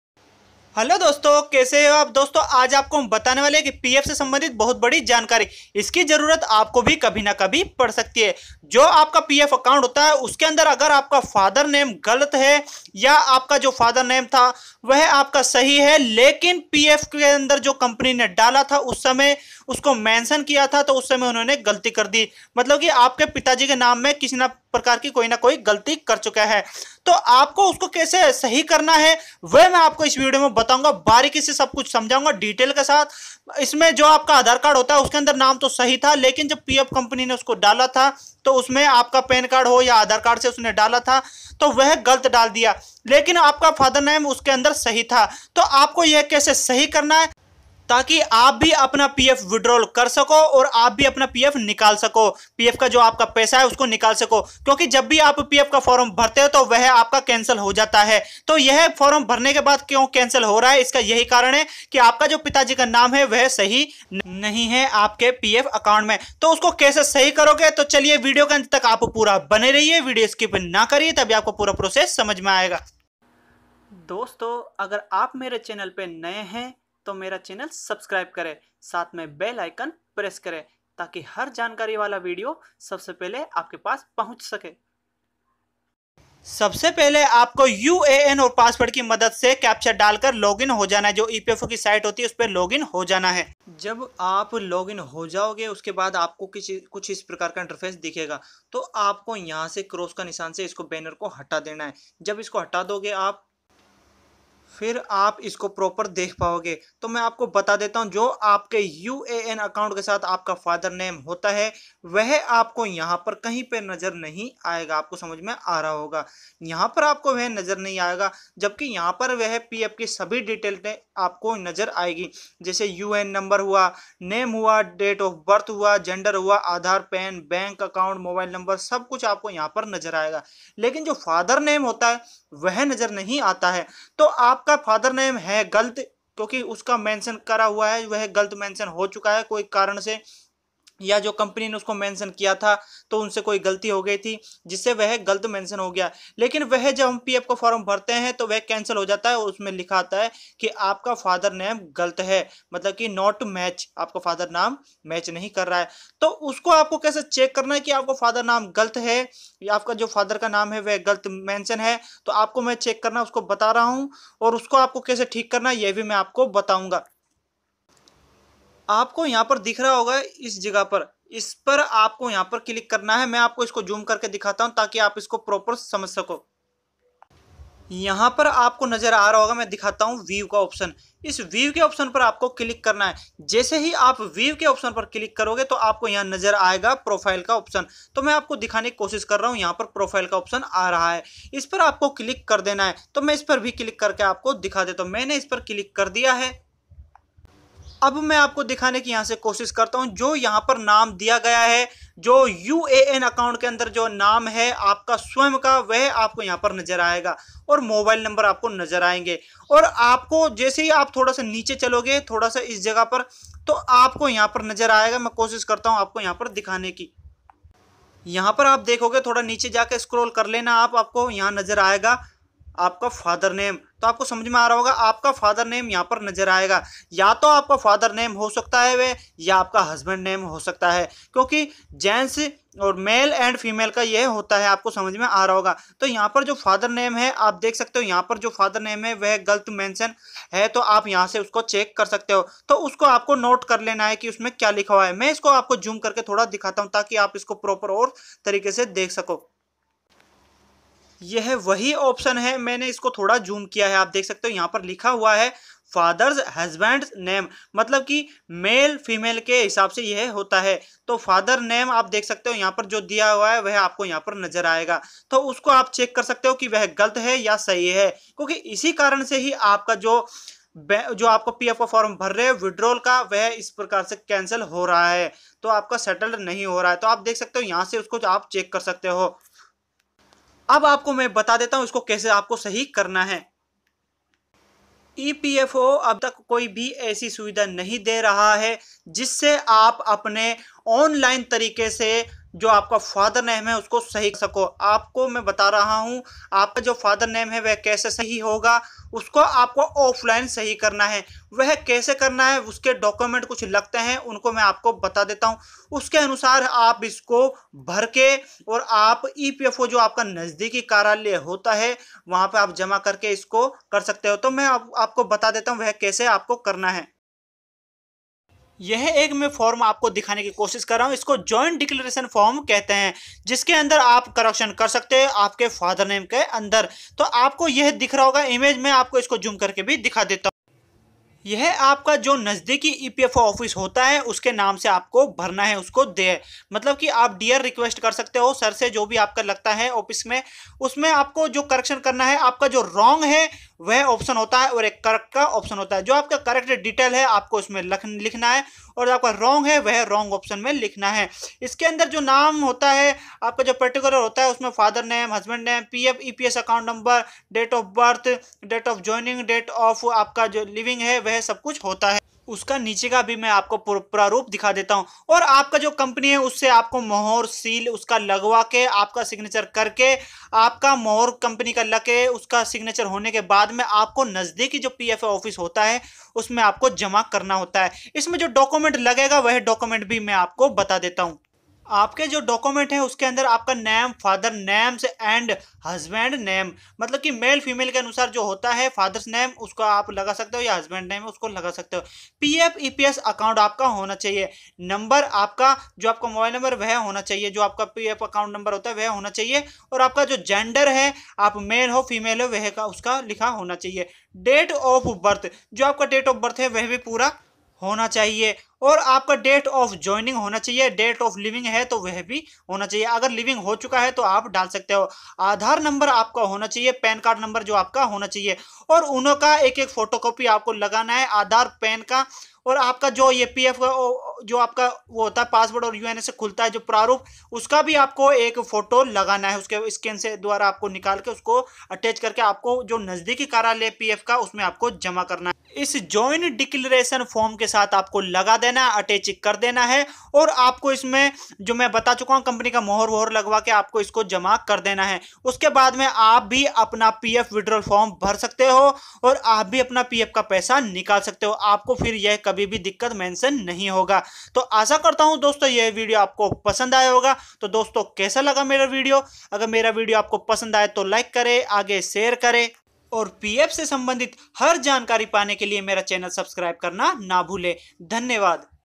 How to correct father name in epf account online || pf me father name kaise sahi kare |pf correction
इस video ke madem se aap apna pf account me jo father name galt hai us ko sahi kar sakte ho usme correction kar sakte ho or kaise sahi karoge us ke liye aap Adhik jankari ke liye video dekho or siko ok thanks 🙏 🙏🙏🙏🙏🔥🔥

pf smbndhit or siko video link👉